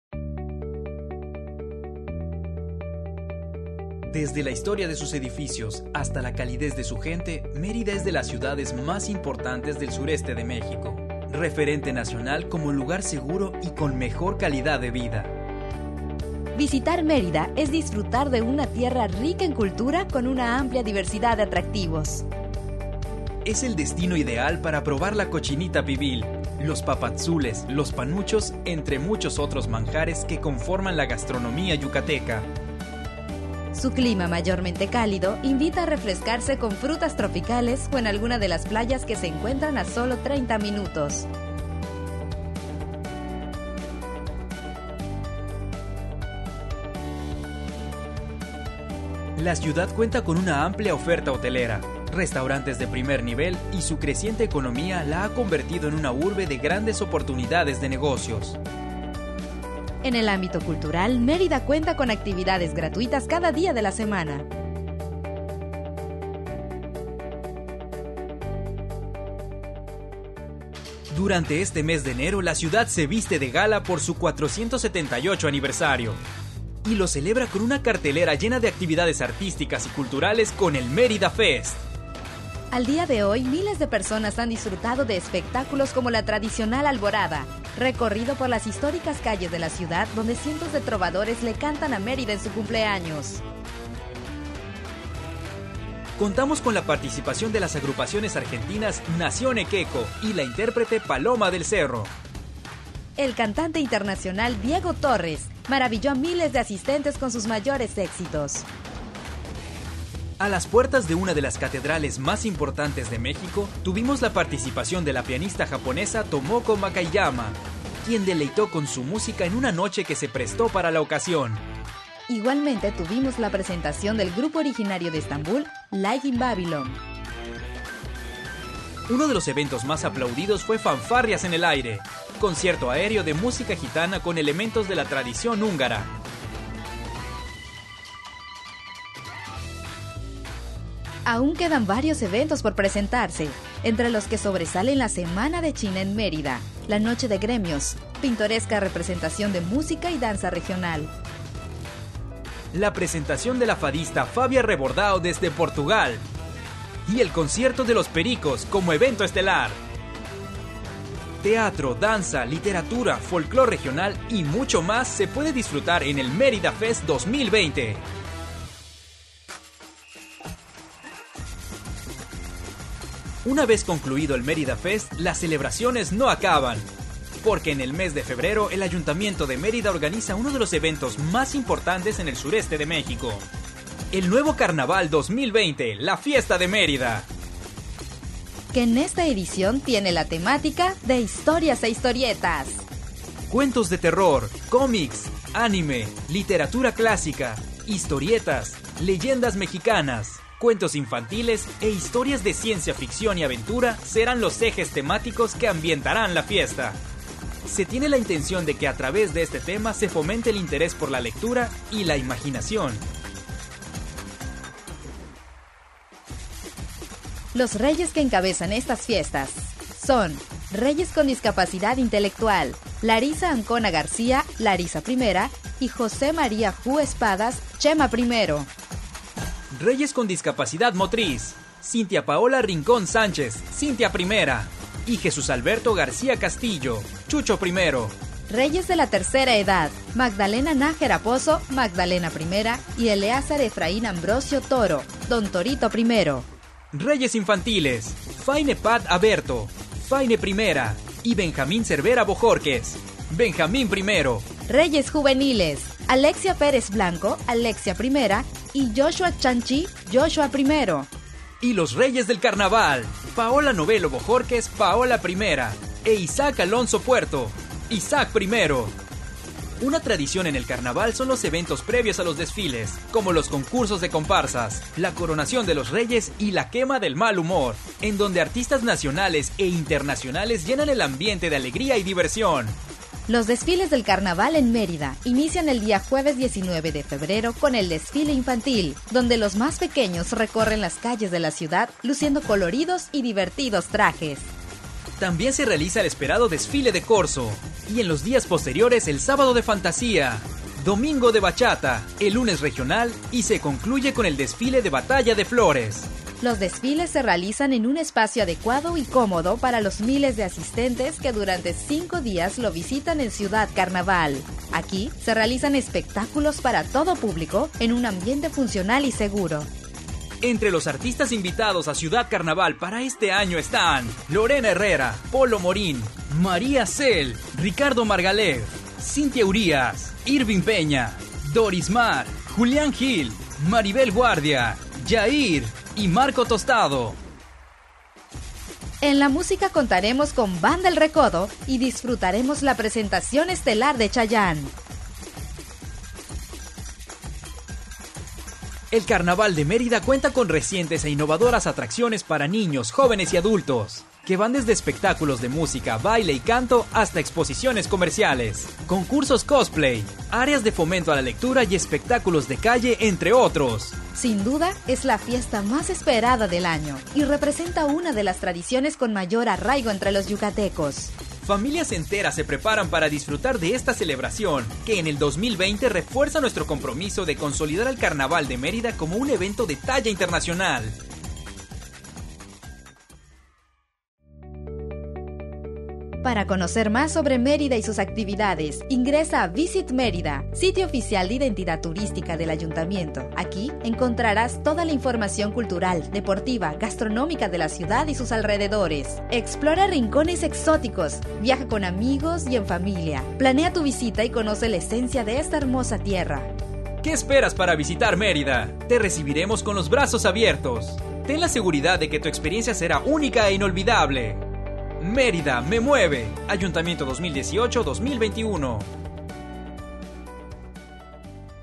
"Mardi Gras" El nuevo carnaval de la fiesta de Mérida 2020 | Mérida Fest | Carros Alegóricos.
¡SE VIENE EL CARNAVAL DE MÉRIDA!

En conferencia de prensa nos platicaron mas de este carnaval, El Carnaval de Mérida es la fiesta más esperada del año y representa una de las tradiciones con mayor arraigo entre los yucatecos.
Entre las innovaciones de este año destaca que Ciudad Carnaval, recinto sede de este evento, estará dividida en cuatro zonas temáticas con opciones para todos los gustos: Tierra de la Algarabía, con más de 12 atracciones para toda la familia; Tierra de la Fantasía, con área de juegos y espectáculos para niños; Tierra de la Fiesta, un concepto inspirado en la celebración del “Mardi Gras” con música, collares multicolores, activaciones comerciales y muchas sorpresas, dirigido principalmente a adultos jóvenes; y la Ruta de la Diversión, con desfiles de comparsas y carros alegóricos.

En este 2020, con el tema “De historias e historietas”, presenta nuevo concepto lleno de zonas de esparcimiento y entretenimiento, así como nuevos espectáculos producidos especialmente para La Fiesta de Mérida y una atractiva cartelera de conciertos que completan Banda El Recodo, Grupo Cañaveral, Edwin Luna y la Trakalosa de Monterrey, así como Moderatto y El Gran Silencio.

Consulta el calendario de actividades y el programa completo del Carnaval de Mérida 2020